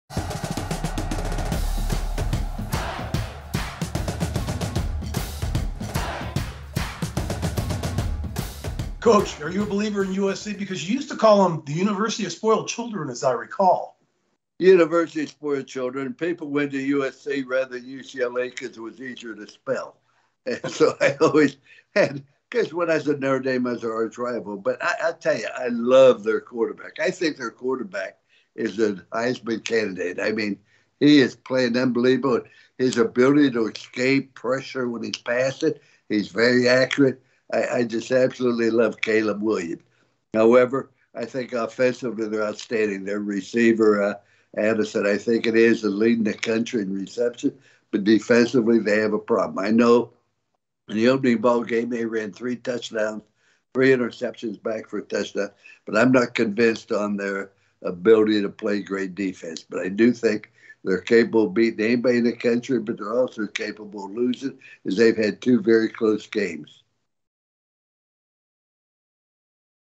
Is USC for Real this Season? - Coach Lou Holtz And Mark May College Football
Lincoln Riley has the Trojans sitting undefeated and squarely in the top 10. Is that enough to get Coach Holtz on board with this team?